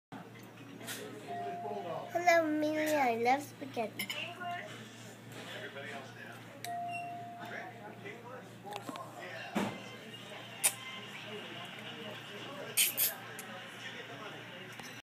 Hello Amelia
Abby wants to show you the mess she makes eating spaghetti.